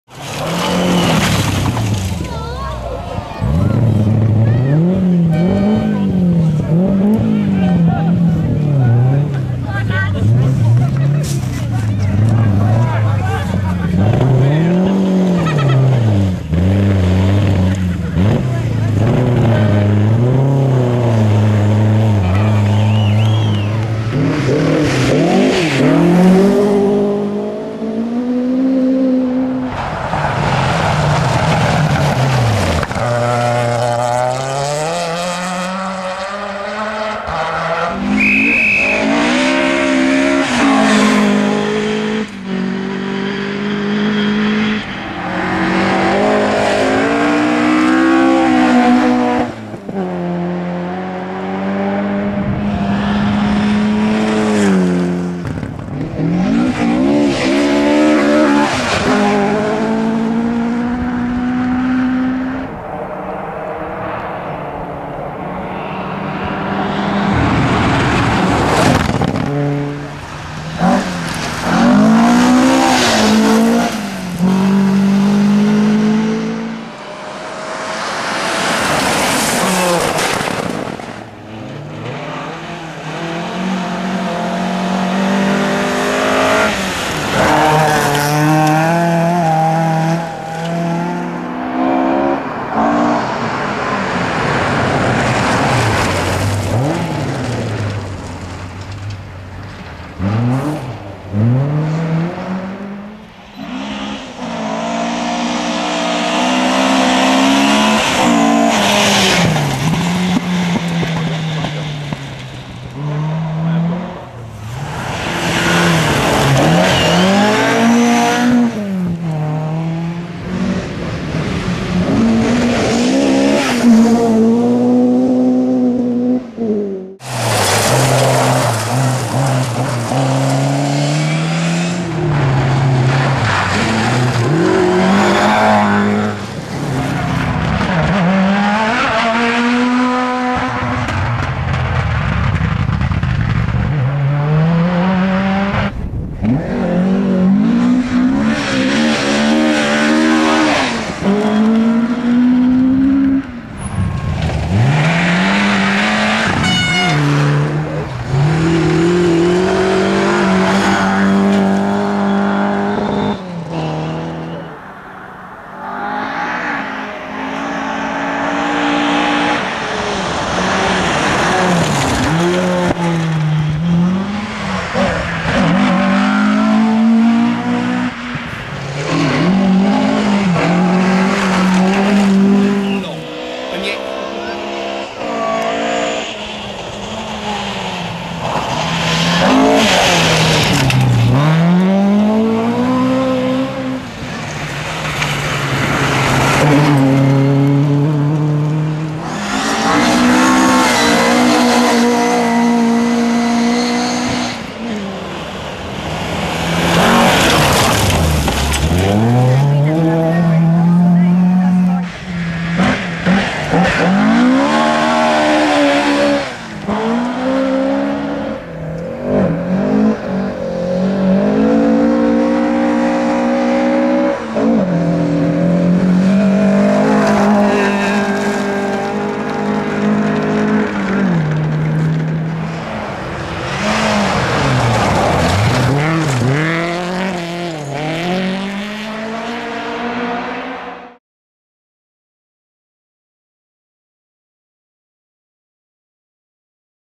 Rallye Sunseeker Escort Special
From the archives comes a 'Best of' video featuring Escorts of all shapes and sizes.